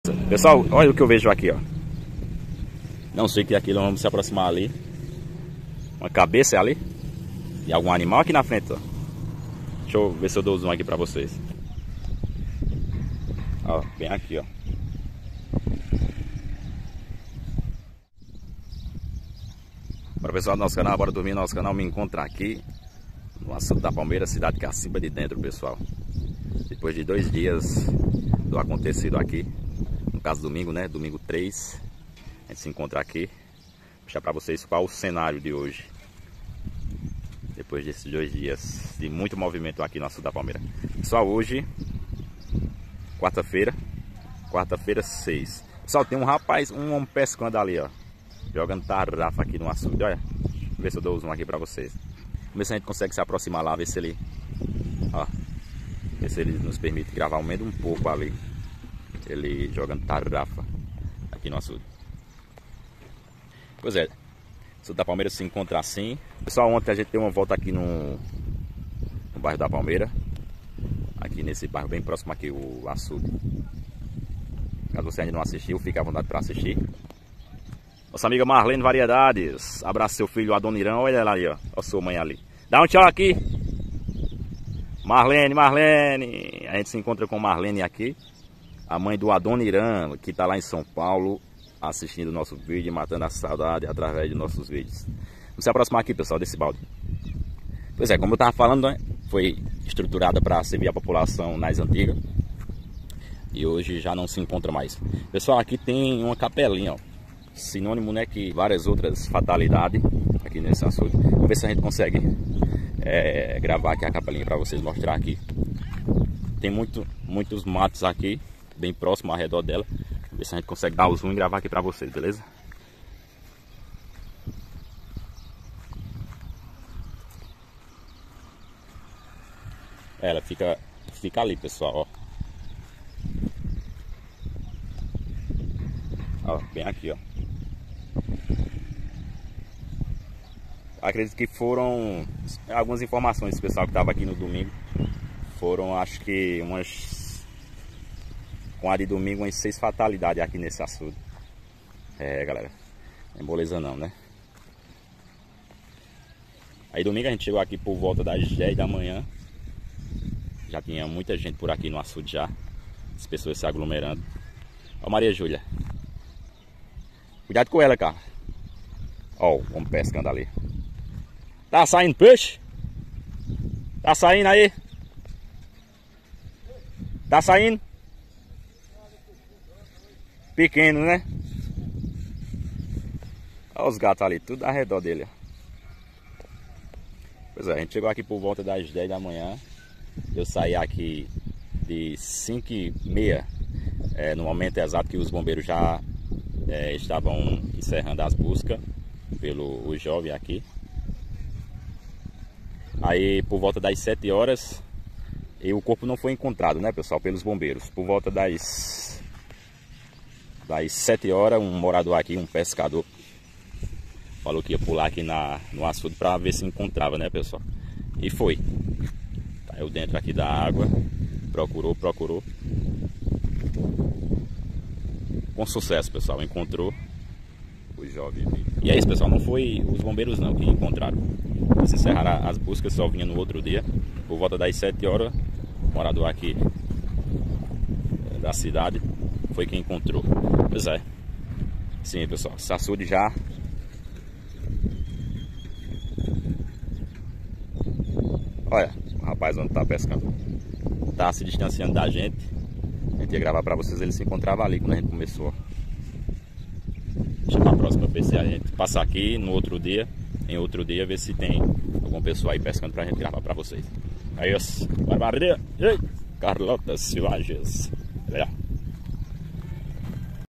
NO AÇUDE DA PALMEIRA JÁ MORRER4M 7 PESSOAS VITIM4 DE AFOG4MENTO - CACIMBA DE DENTRO, FUI LÁ MOSTRAR
Açude da Palmeira em Cacimba de Dentro PB, VITIM4 DE AFOG4MENTO